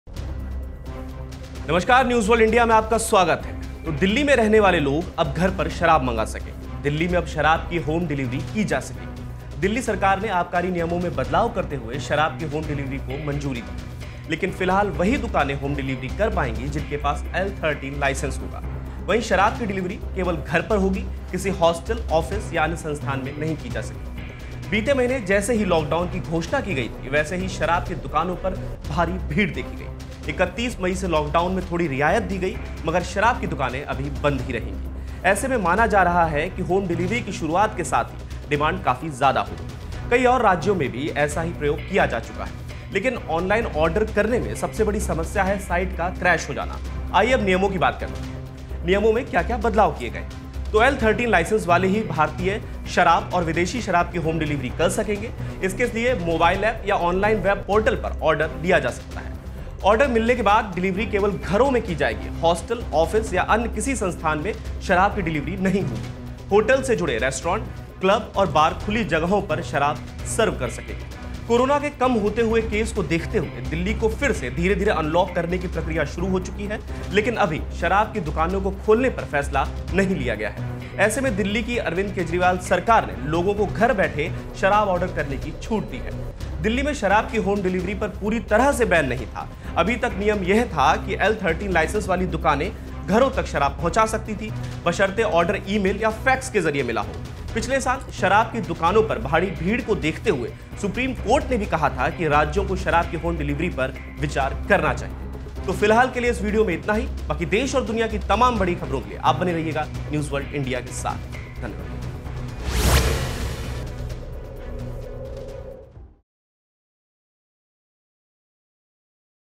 Delhi govt allows home delivery of liquor through an app, website । Online order alcohol
About This Video ►दिल्ली सरकार ने अभी तक 'शराब ऑनलाइन कैसे ऑर्डर करें' के संबंध में कोई स्पष्टीकरण नहीं दिया है। लेकिन कुछ वेबसाइट और प्लेटफॉर्म ऐसे भी हैं, जिन्होंने पूर्व में ऑनलाइन शराब डिलीवरी की सेवा प्रदान की है।
पिछले साल, पश्चिम बंगाल, ओडिशा, महाराष्ट्र आदि राज्यों ने Amazon, BigBasket, Zomato, और Swiggy जैसे प्लेटफार्मों के माध्यम से शराब की ऑनलाइन डिलीवरी की अनुमति दी थी।
The Delhi government has not yet provided any clarification regarding ‘how to order liquor online’. But there are some websites and platforms who have in the past, provided the service of online liquor delivery.
Last year, states like West Bengal, Odisha, Maharashtra, etc allowed online delivery of liquor through platforms like Amazon, BigBasket, Zomato, and Swiggy